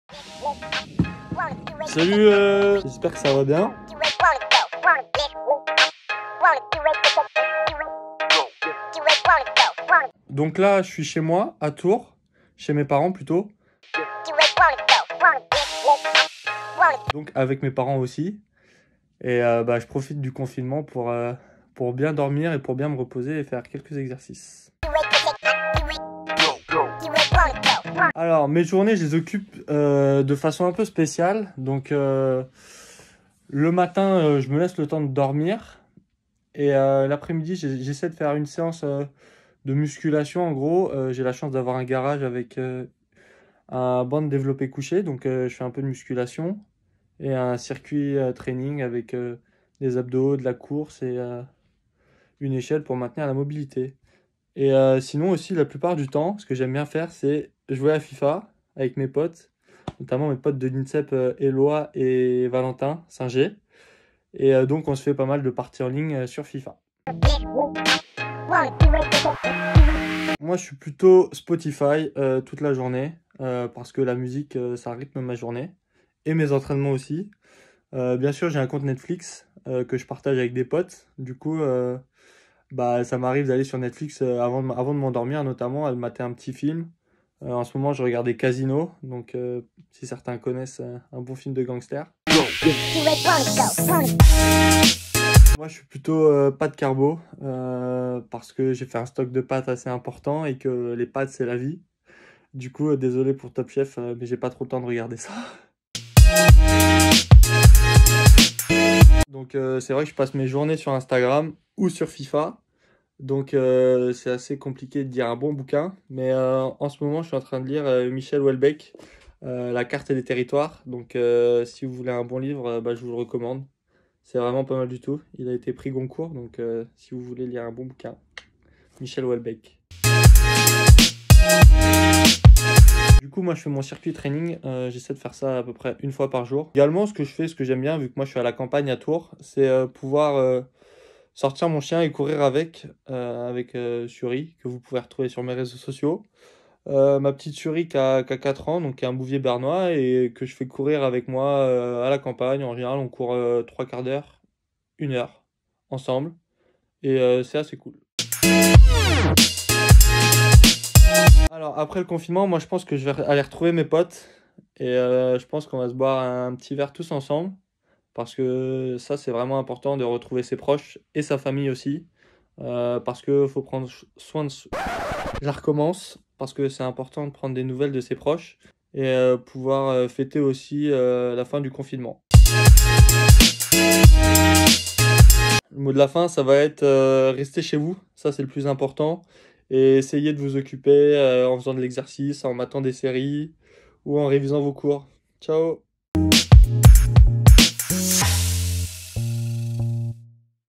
GameofHome - Lucas MAZUR
Entre les circuits training, les parties sur FIFA ou encore la lecture, Lucas MAZUR a trouvé de quoi bien s'occuper en cette période !